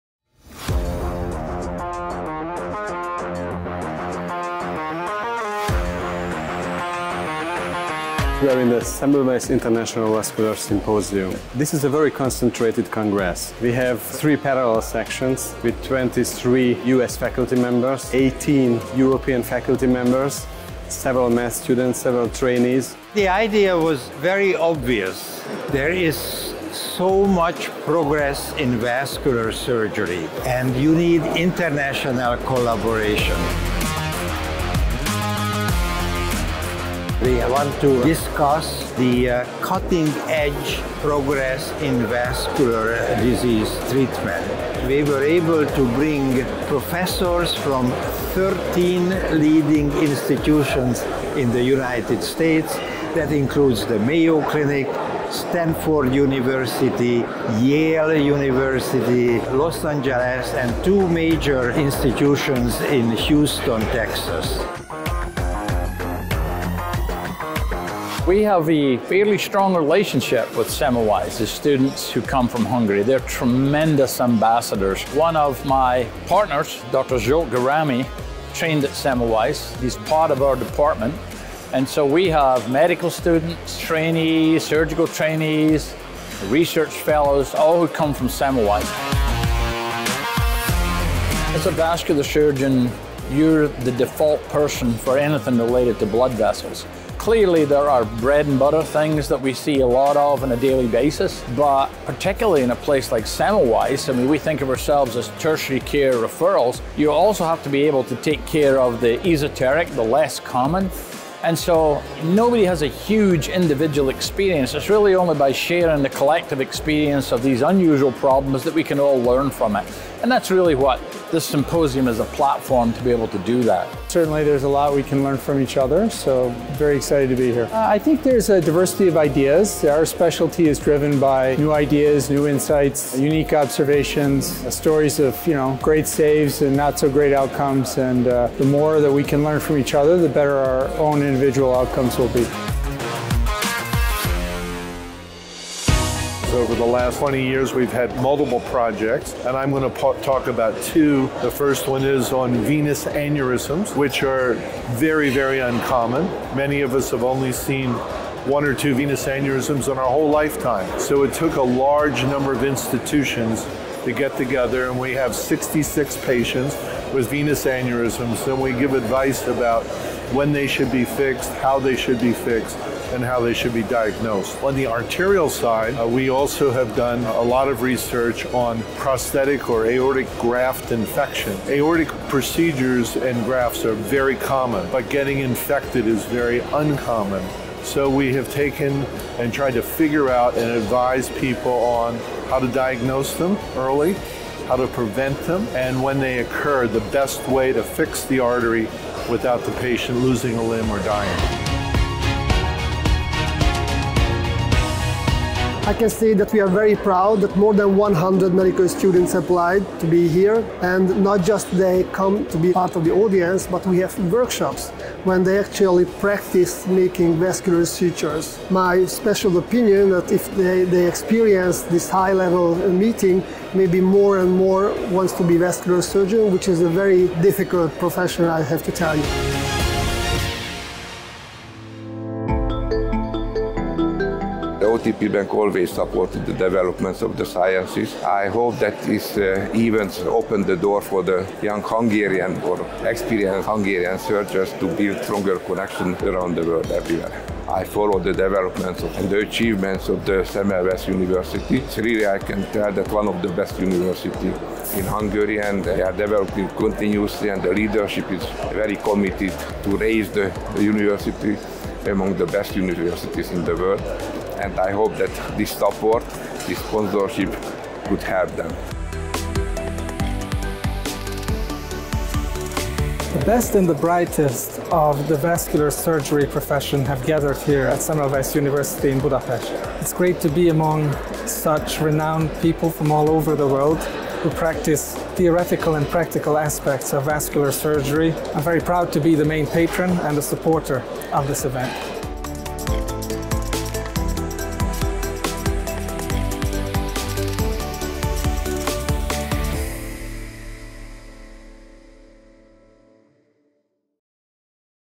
Semmelweis International Vascular Symposium 2023
A 3-day international vascular symposium was organized jointly by Semmelweis University and the Frank J. Veith International Society. The meeting brought together an outstanding international faculty from medical institutions in Europe and the United States on 18-20 May to discuss the latest developments in the medical, endovascular and surgical treatment of vascular diseases.